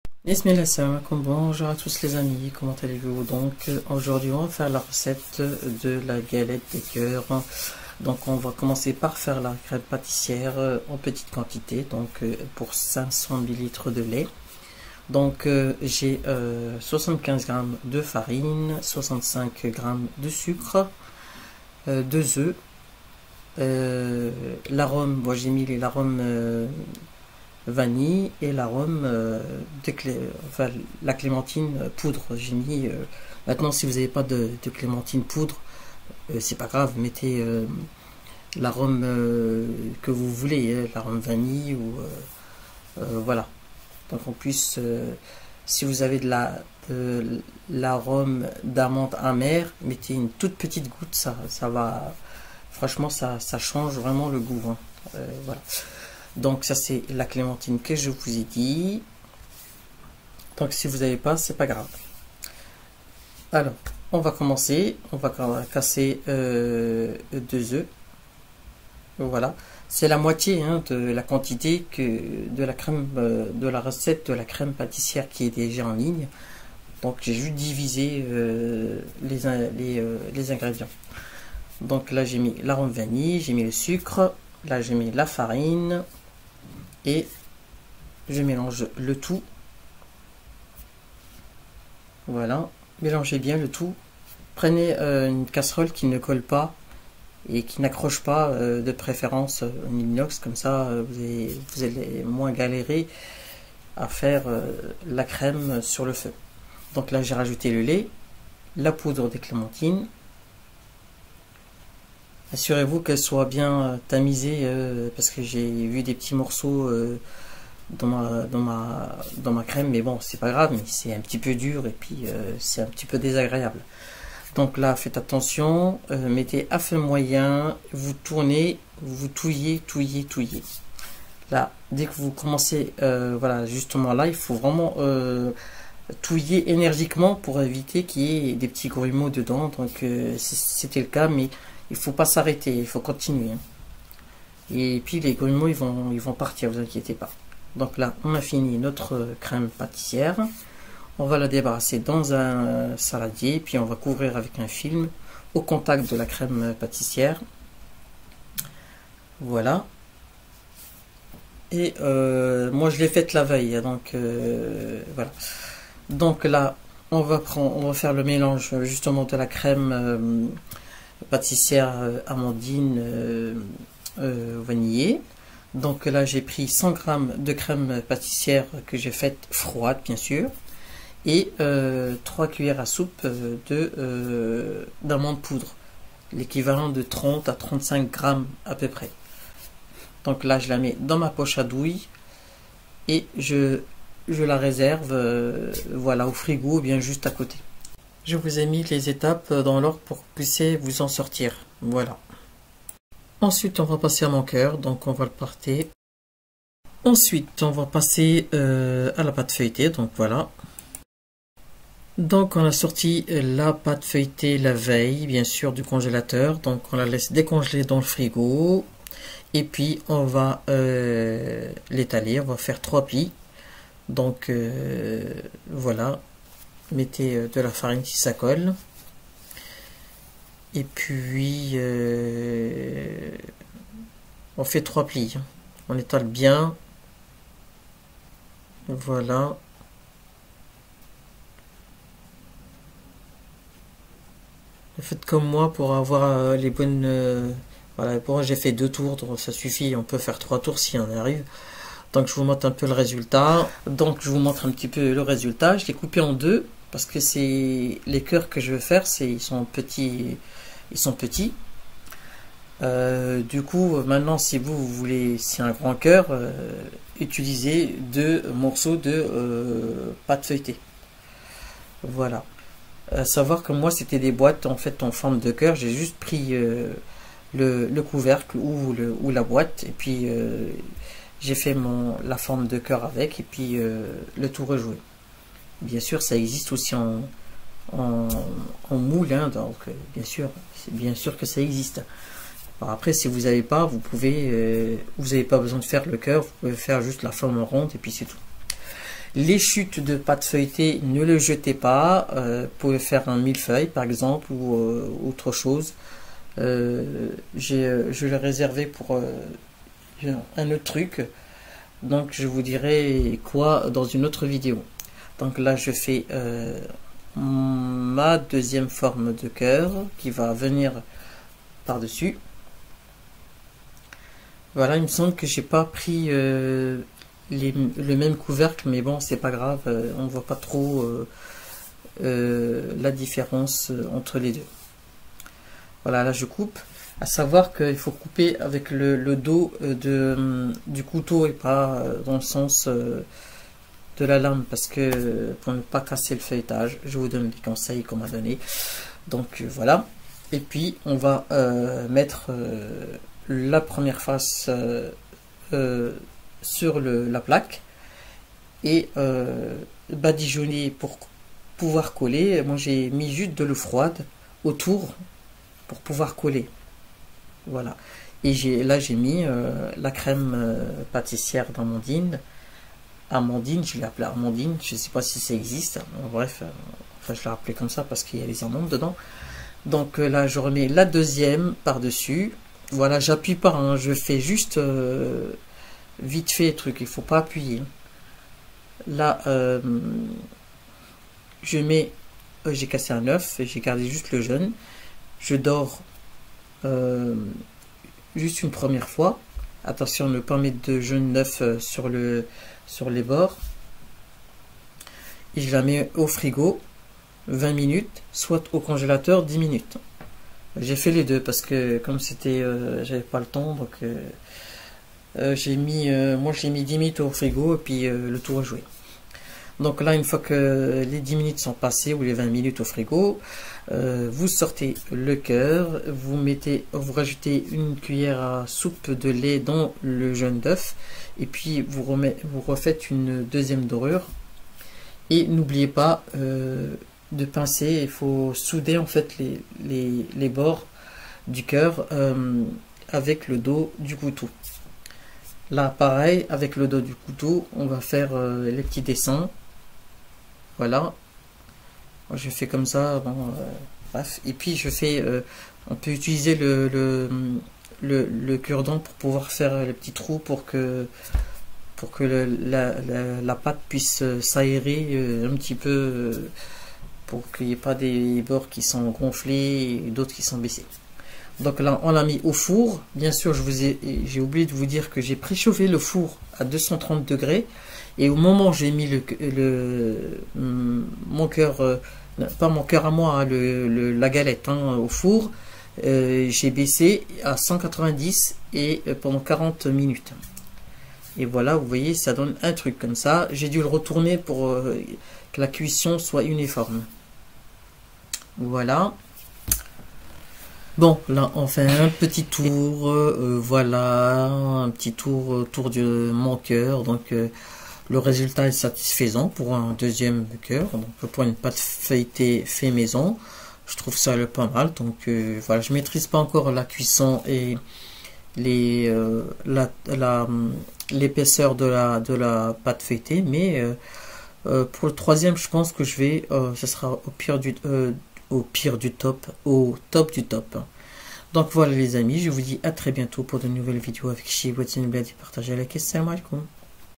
Galette des cœur pour les bons cœurs !/ The recipe! Galette en cœur for the good hearts!
Galette des rois revisité! Avec la pate feuilletée fait maison !
Remarque pour la 2 -ieme  dorure  c'est pas 1 cuillère a soupe mais 1 cuillère a café ! je me suis tromper , c'est ce qui arrive a ce qui ne s'écoute pas ! lol !
1 er parti la crème pâtissière en petite quantité
2 -ème la crème d'amandine
3eme flaconnage de la galette en cœur
4 cuisson
préchauffer le four a 230 °
puis pour la cuisson de la galette baisser a 190°
cuisson 40 minutes environ